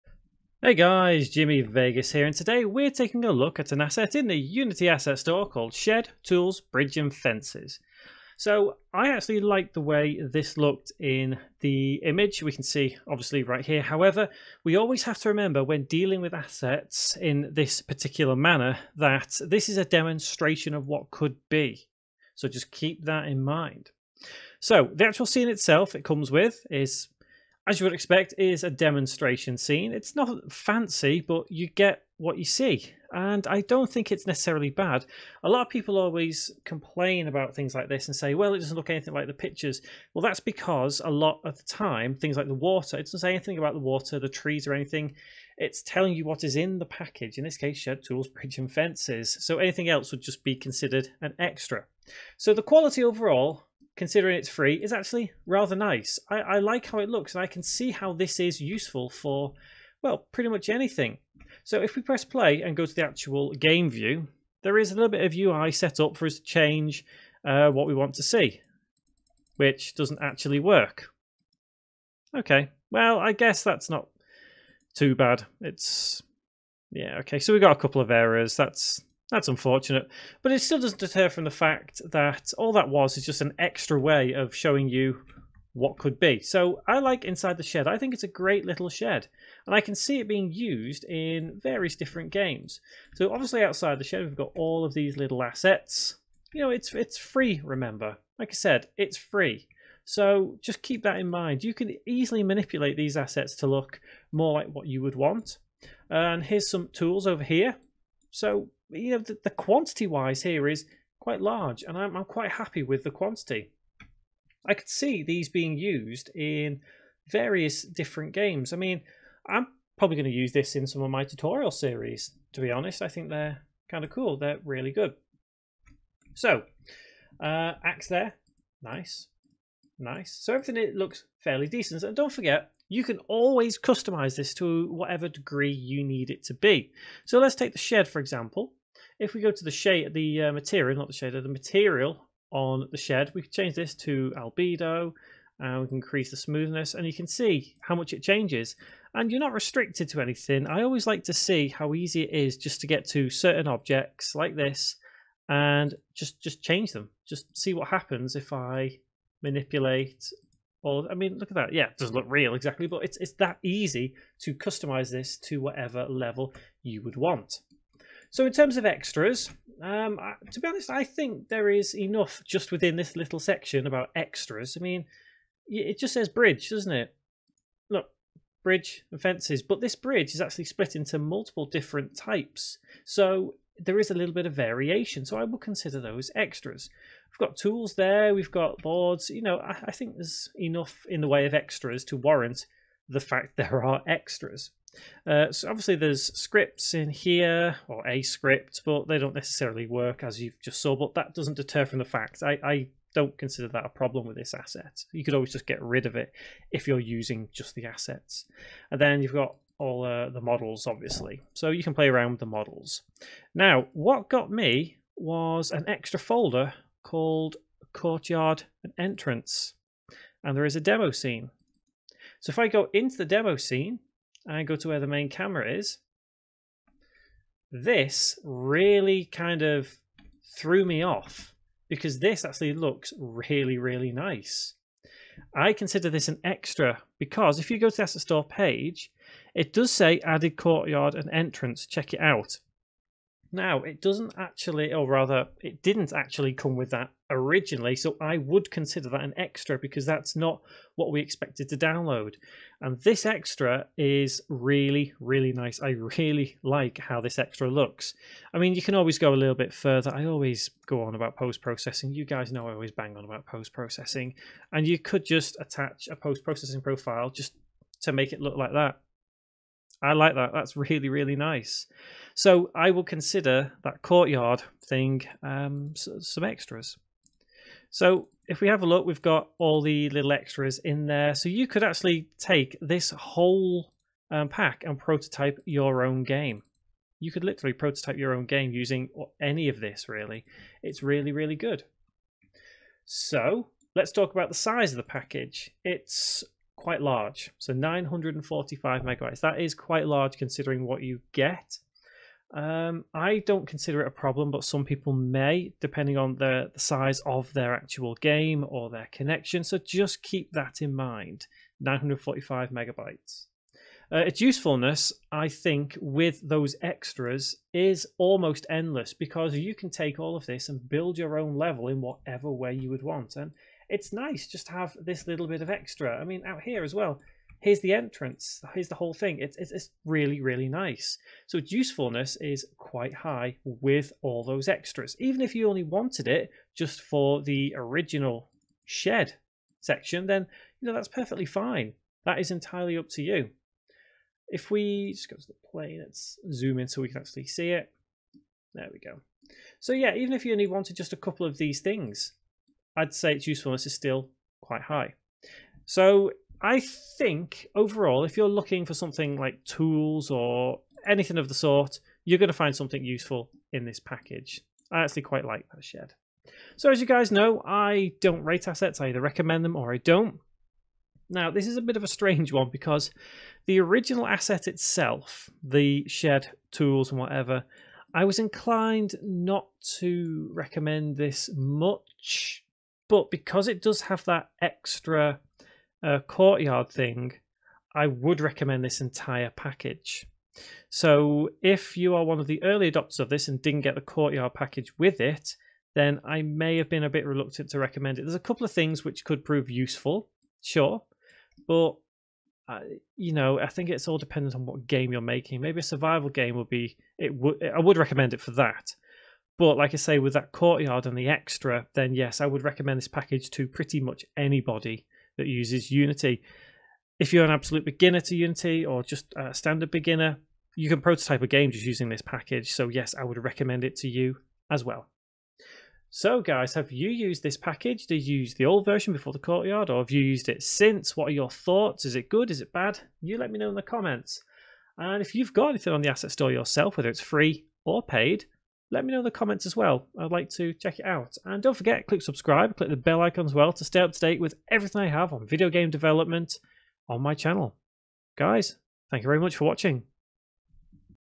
UNITY ASSET REVIEW | SHED, TOOLS & BRIDGES | INDEPENDENT REVIEW BY JIMMY VEGAS ASSET STORE
This is a package containing several outdoor props such as varied fences, prefabed bridge parts, a shed and many tools in it. Review conducted independently and no money was received for this review.

Who Is Jimmy Vegas?